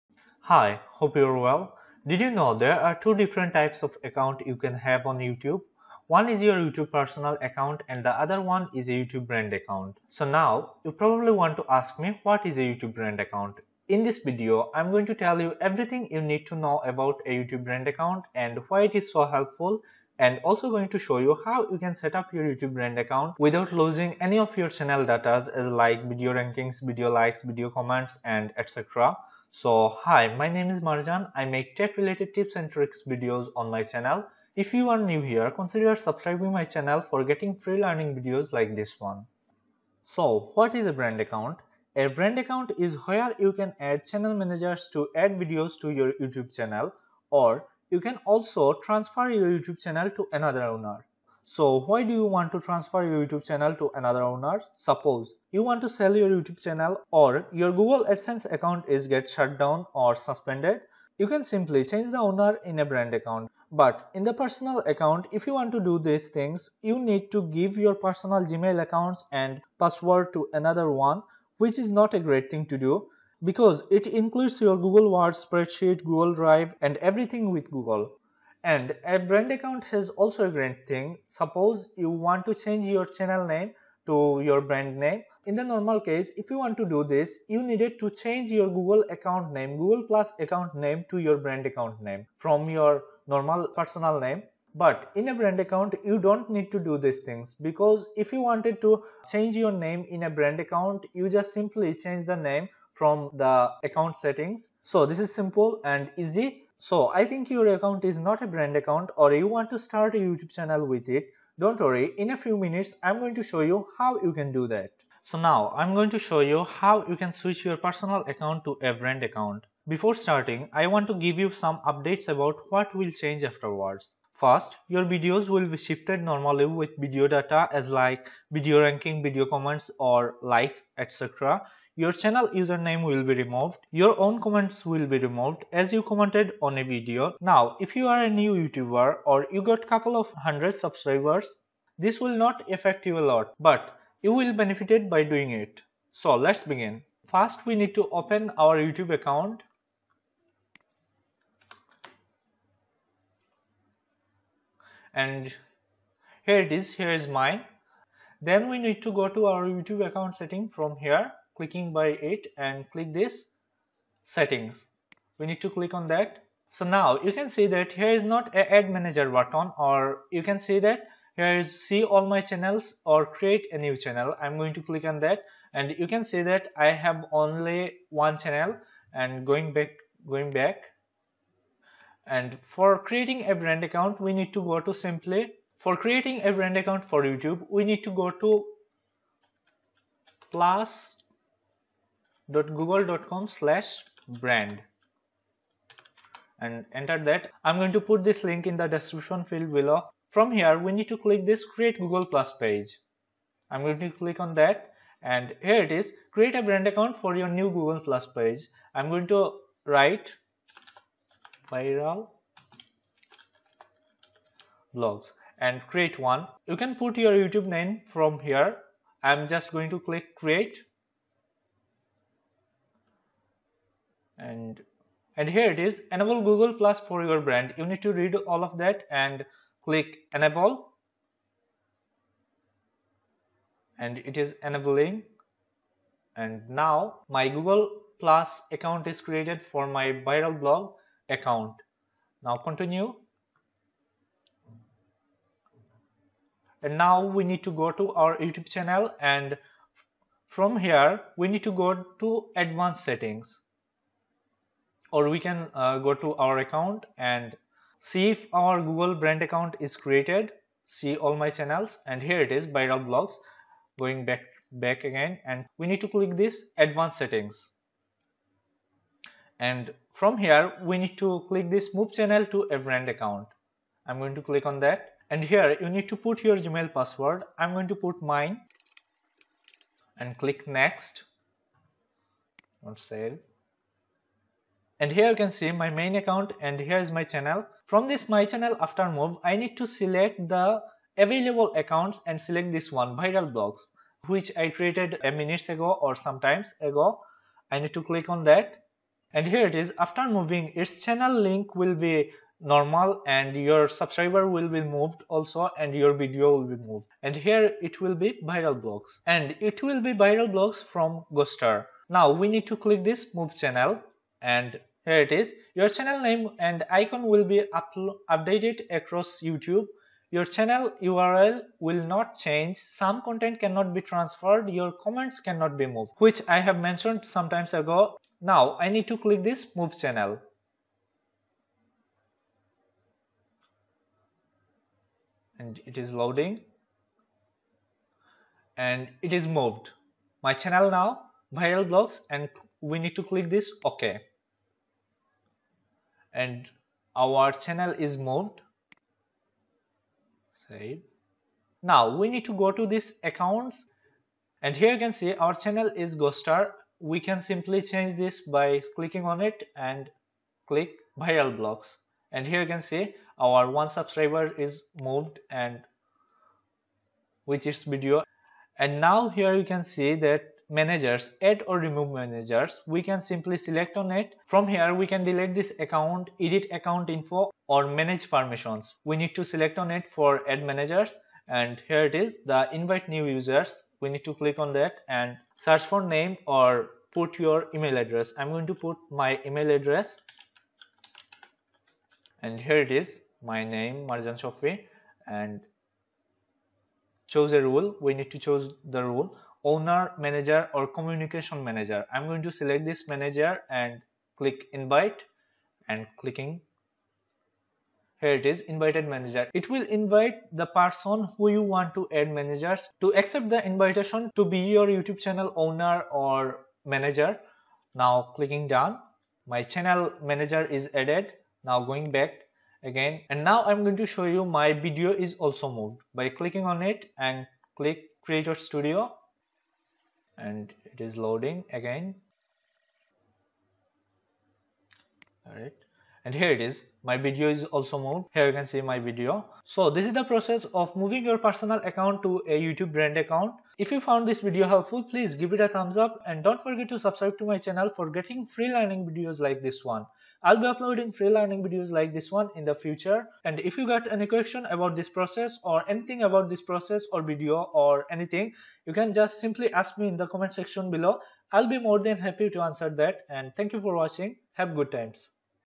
Everything About YouTube Brand Account! How to Setup a Brand Account
Everything You Need to Know About YouTube Brand Account | How to Setup Brand Account - TechChannel

There are some of us who didn't know about the types of YouTube accounts. But in this video, I'm going to show you The Two Types of YouTube Accounts One is our Personal Account and the other one is A YouTube brand account.

Normally YouTube Brand account is used by Brands or Big creators to manage their YouTube channel more easily. But we can also use it for our own Channels.

By using Youtube Brand Account We can add managers to our YouTube channel to add videos and modify them, and also we can add channel communication managers. And The great thing about a YouTube Brand Account is We can change the owner of a channel very easily.

There are several other helpful things that we can use by setup a youtube brand account!

For full understanding please watch till the end.

Get more helpful tech videos regularly.

Marjan Shofy